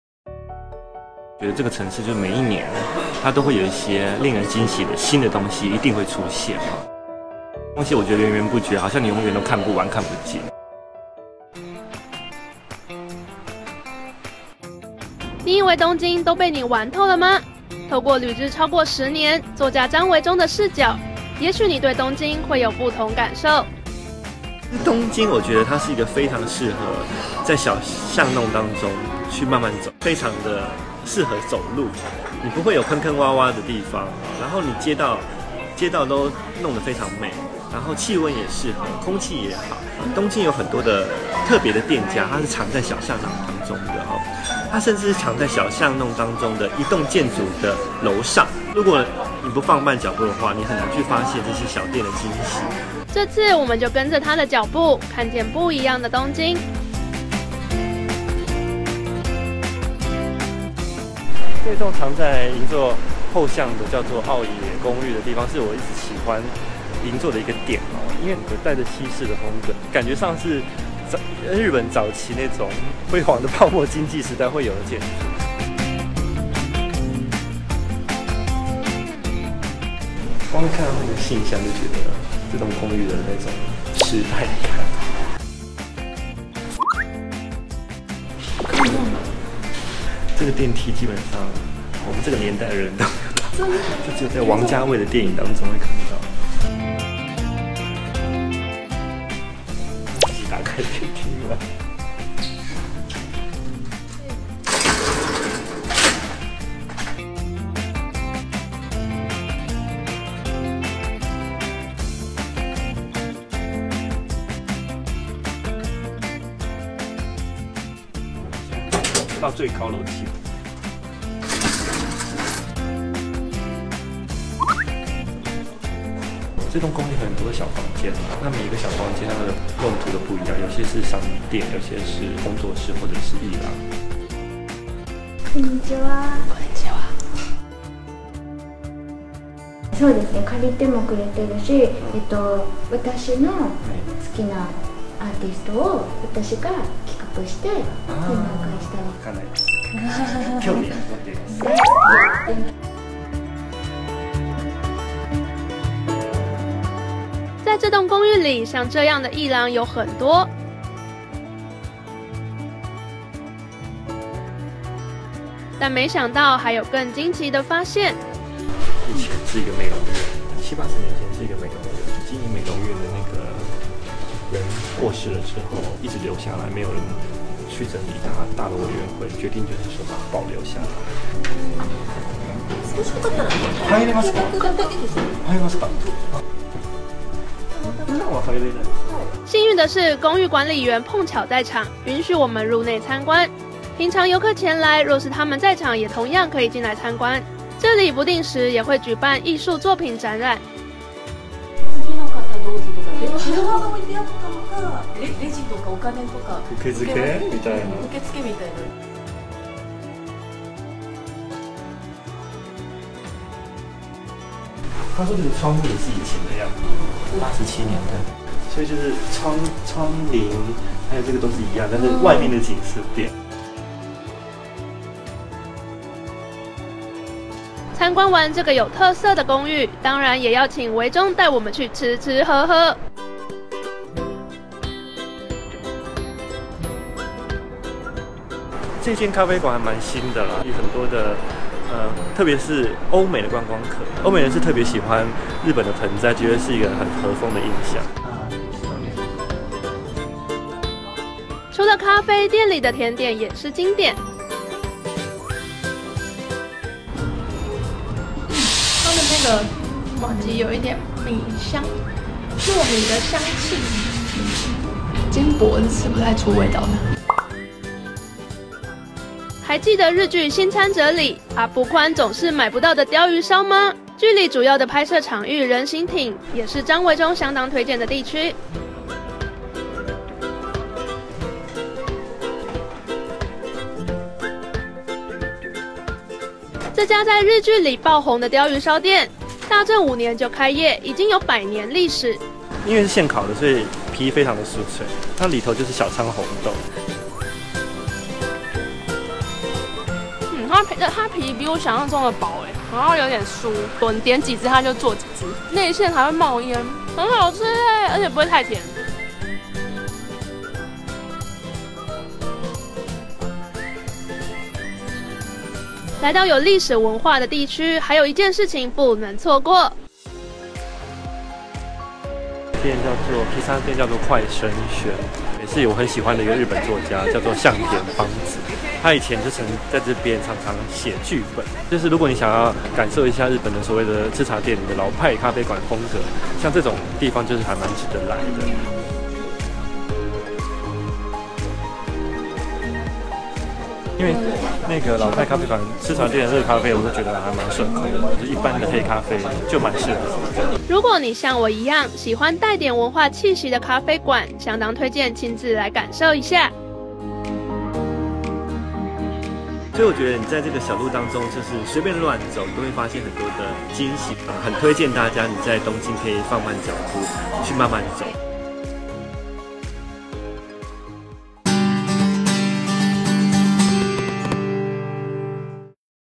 【達人帶路】東京小路「亂撞」復古公寓 吃阿部寬買不到的鯛魚燒 | 蘋果新聞網
東京，多少旅人熟悉的城市。近年廉價航空興盛，往返便利的東京，或許不再是旅客遊日的首選，但在旅日作家張維中的眼裡，這座城市帶給他的總是驚喜、巷弄裡總有新發現。「每一年它都會有令人驚喜、新的東西一定會出現，源源不絕，好像你永遠都看不完、看不盡。」張維中旅居東京超過十年，記者對他眼中充滿驚喜的東京感到好奇，這次索性請他帶路、引領讀者深入巷弄窄巷裡，挖掘東京故事。